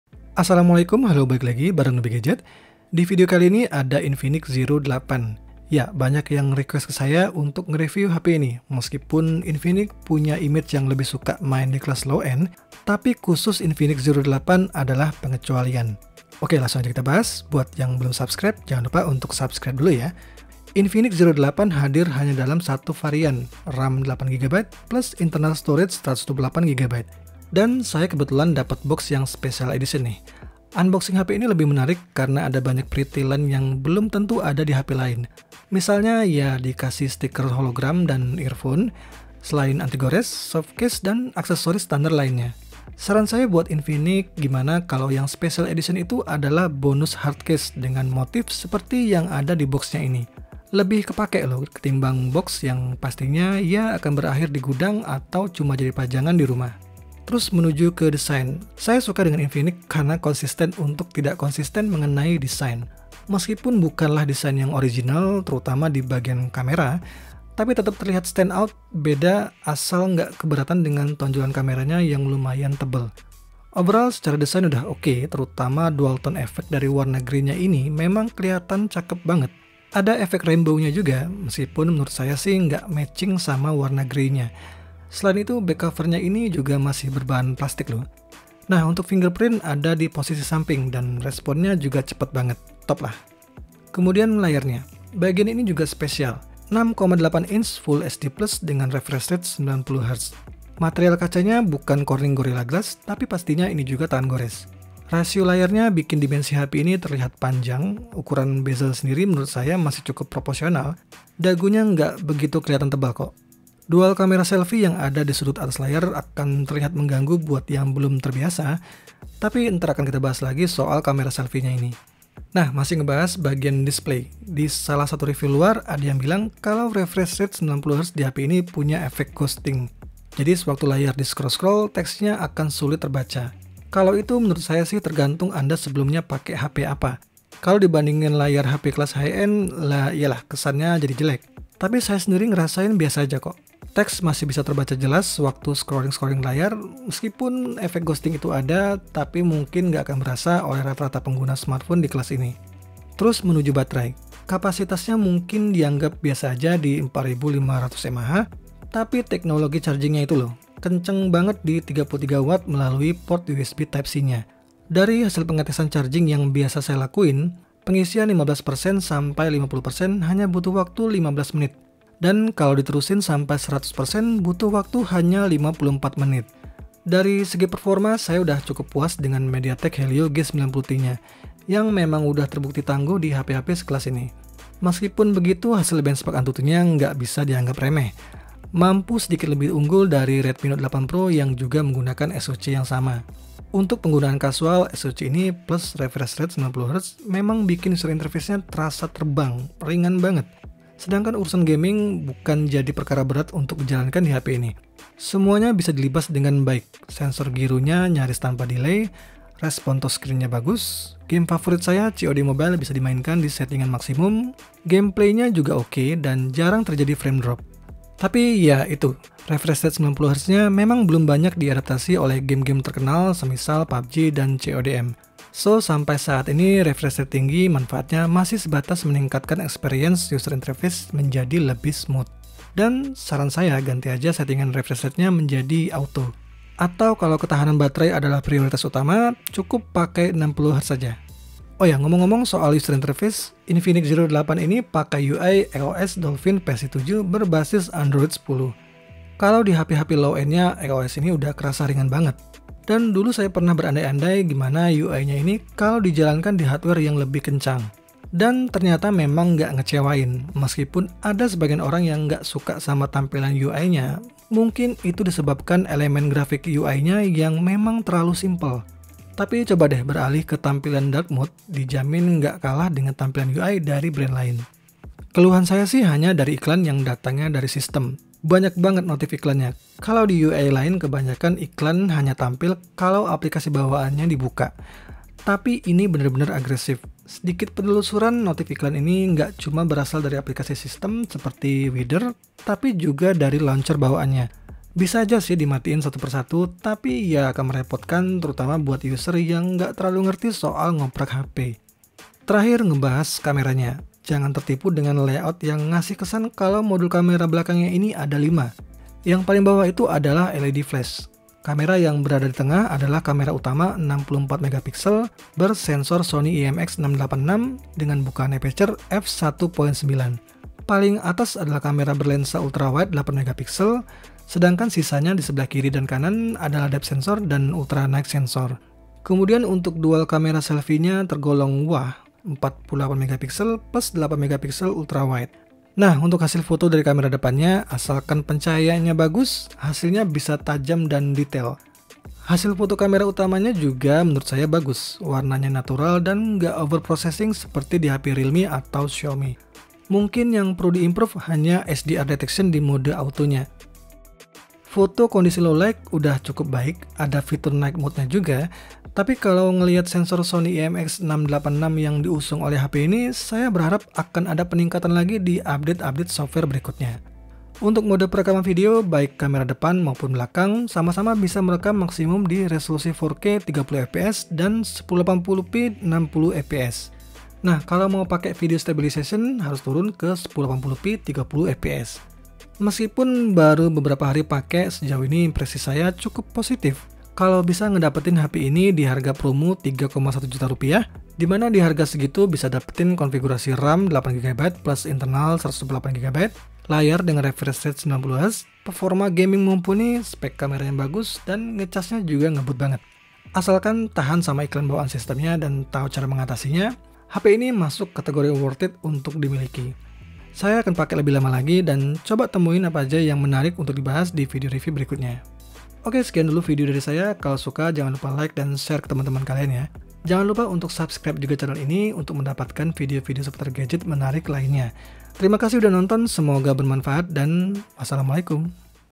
Infinix Zero 8 Review - Part 1
Ini dia Infinix Zero 8 Review Indonesia, hape ini punya spesifikasi gaming yang garang, kamera oke, harga hanya 3 jutaan dan skor antutu yang katanyaaa.. bisa melampaui skor Redmi Note 8 Pro.

Gadget bekas review (kalau ada) cek Shopee/Tokopedia : NubiGadget
Follow IG untuk tips dan update gadget yang sedang di-review : @nubigadget